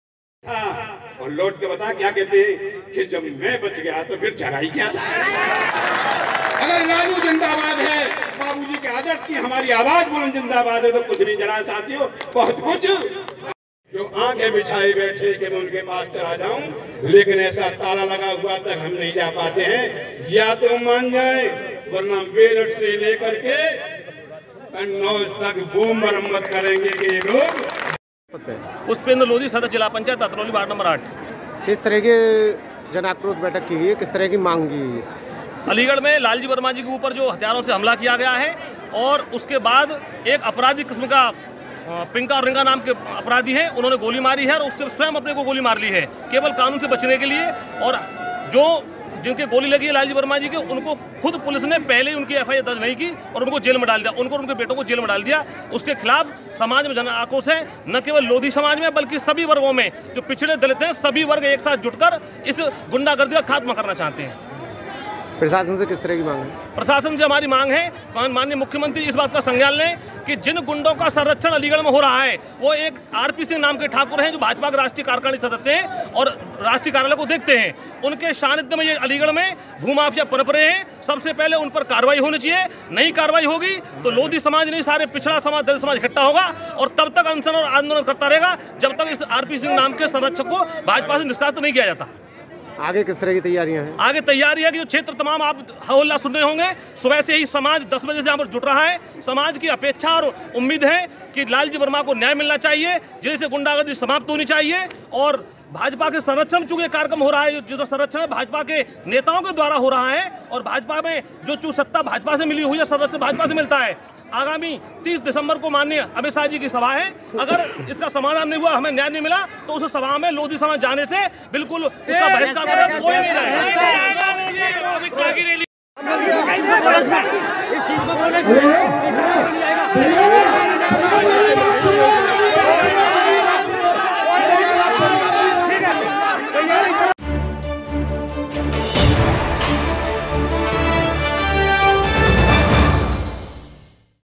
गृह मंत्री अमित शाह की रैली में लोधी समाज नहीं लेगा हिस्सा, दी चेतावनी
30 दिसंबर को अमित शाह की रैली से पहले लोधी समाज के लोगों ने जन आक्रोश जनसभा का आयोजन किया गया लोधी समाज ने जन आक्रोश जनसभा में 30 दिसंबर को अमित शाह की होने वाली रैली को लेकर बहिष्कार कर दिया।इसके साथ ही अमित शाह की रैली में हिस्सा लेने से भी मना कर दिया हैं।अलीगढ़ जिले में लोधी समाज का आरोप है भाजपा पार्टी के एक राष्ट्रीय कार्यकारिणी सदस्य के इशारे पर लालजी वर्मा और उसके बेटों के खिलाफ एकतरफा कार्रवाई हो रही है। पुलिस ने लालजी समेत उसके बेटों को जेल में डाल दिया है। जब तक बीजेपी पार्टी राष्ट्रीय कार्यकारिण सदस्य को पार्टी से निष्कासित नहीं करती तब तक लोधी समाज का जनाक्रोश आंदोलन जारी रहेगा।